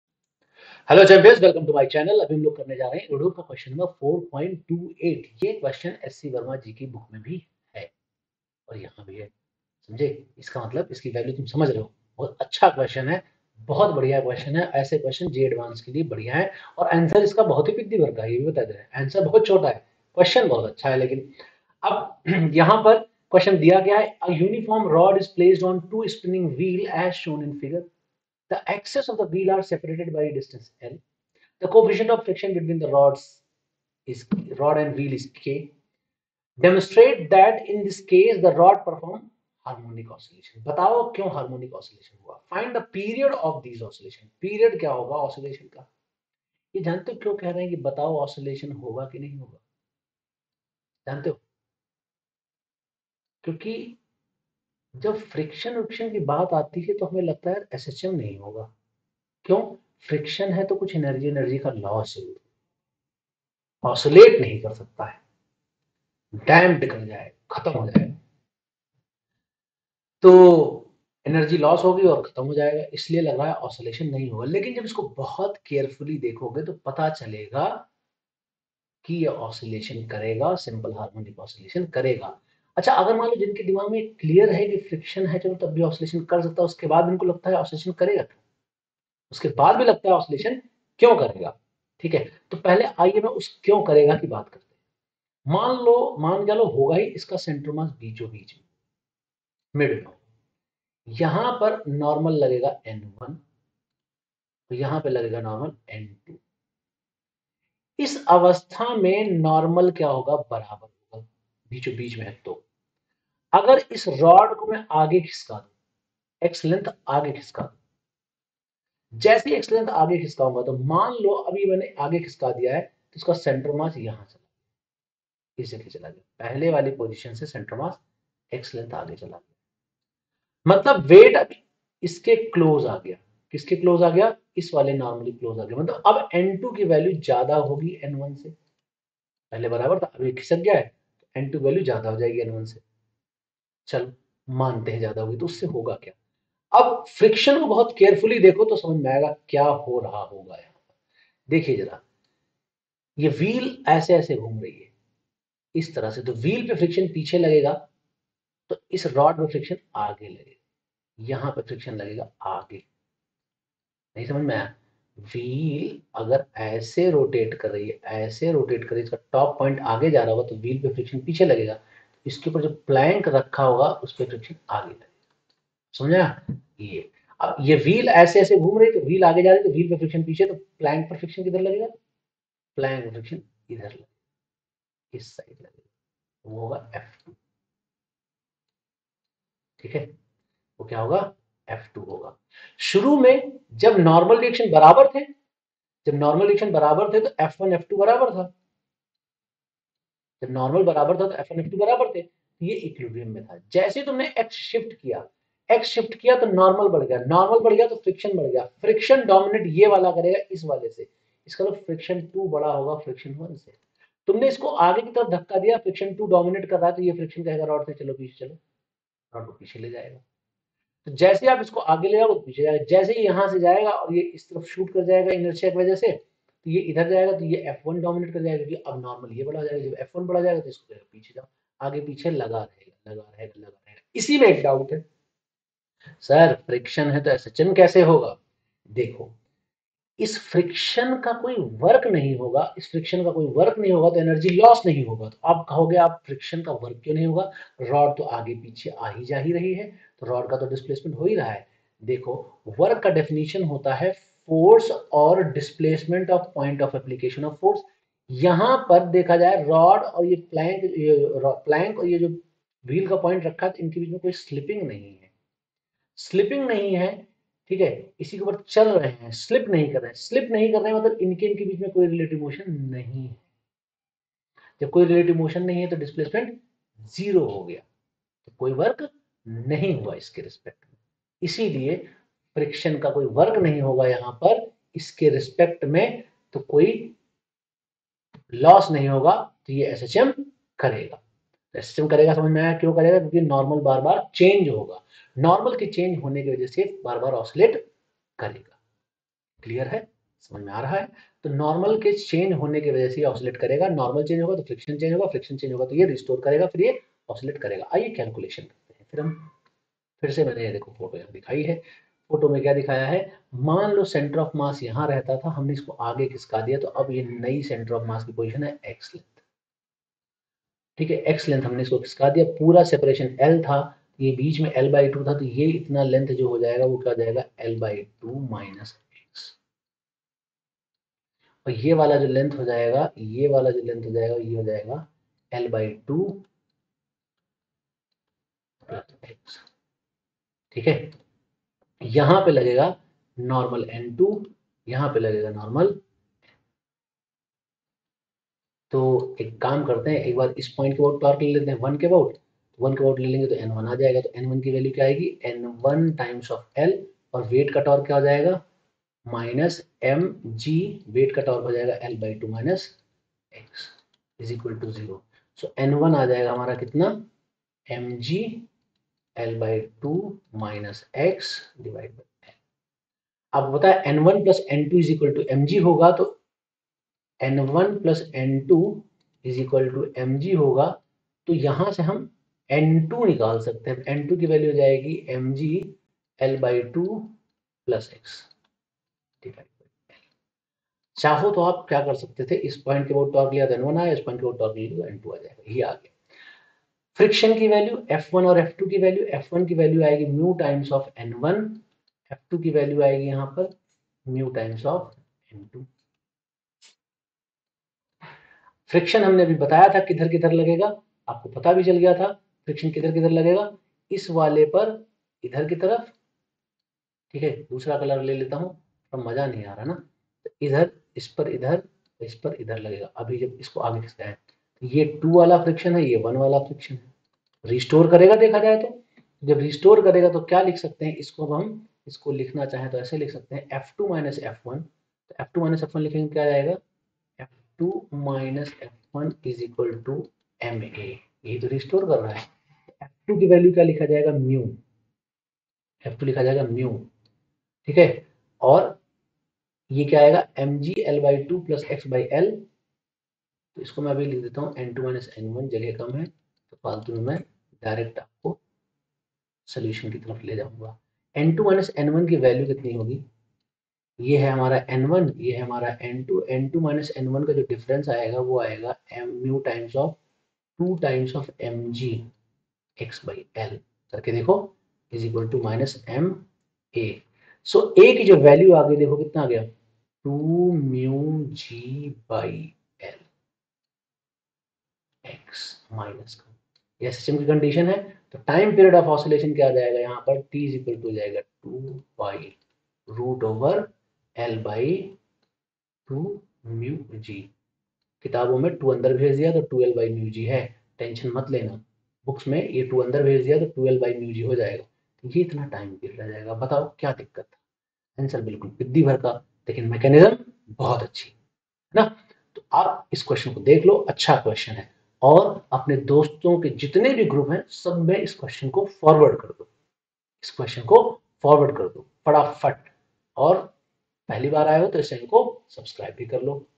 Irodov Solution 4.28 | OSCILLATIONS | Important Questions Irodov | Jee Mains | Advanced | Neet
After watching this video, we are going to learn HOW TO SOLVE IRODOV'S PROBLEMS IN 1 MINUTE.

📚 Send your doubts in the comments.